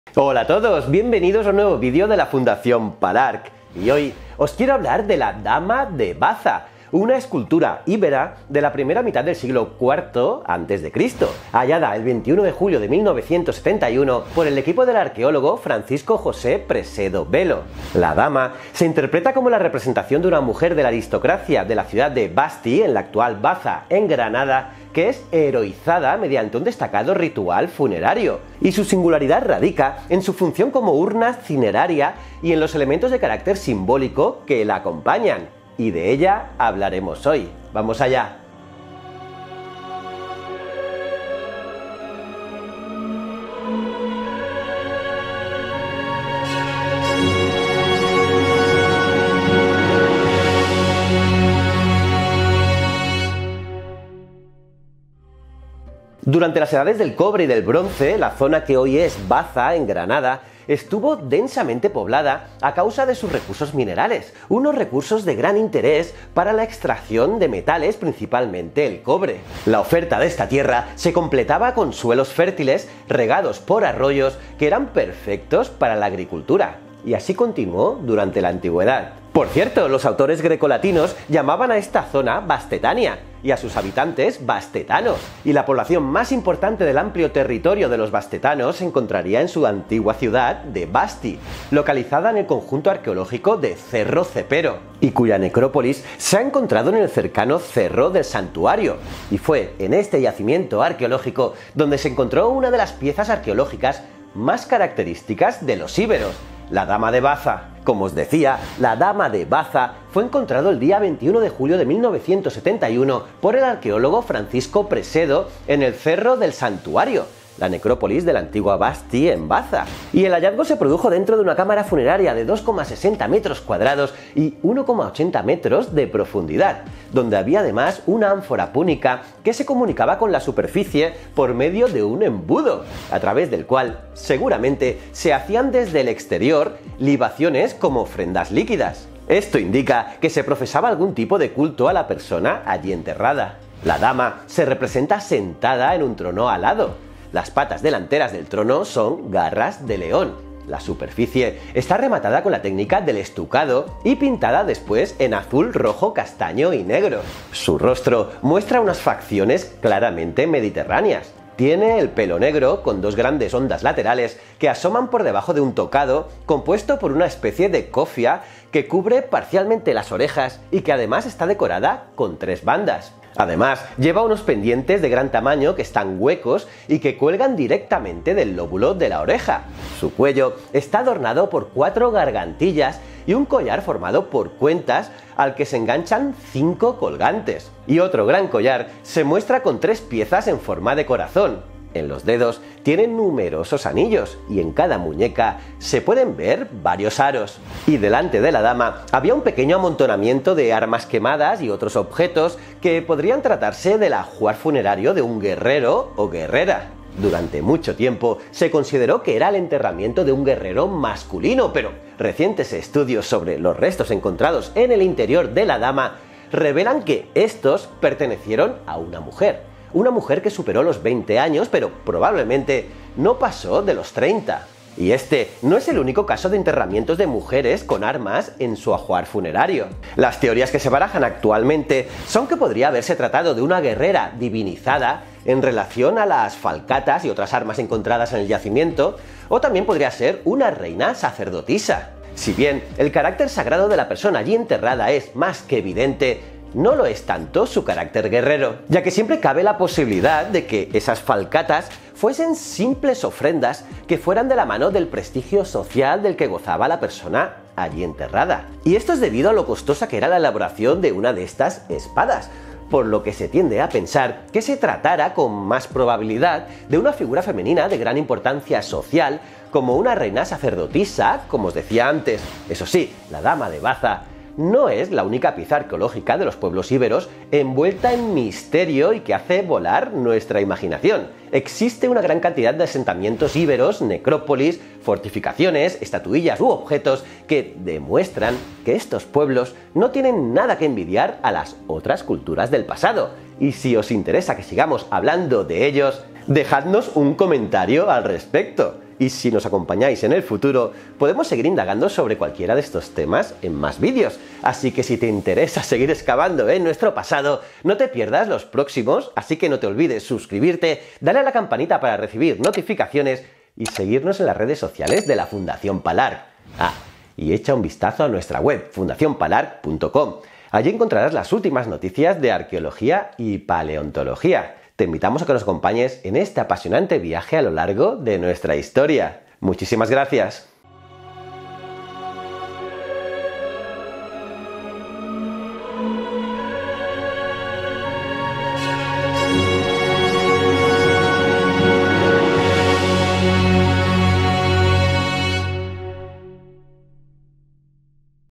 La Dama de Baza | Un símbolo de la Cultura Íbera
Hoy os quiero hablar de la Dama de Baza, una escultura íbera de la primera mitad del siglo IV a.C., hallada el 21 de julio de 1971 por el equipo del arqueólogo Francisco José Presedo Velo. La Dama se interpreta como la representación de una mujer de la aristocracia de la ciudad de Basti, en la actual Baza en Granada, que es heroizada mediante un destacado ritual funerario.  Y su singularidad radica en su función como urna cineraria y en los elementos de carácter simbólico que la acompañan.

Y de ella hablaremos hoy.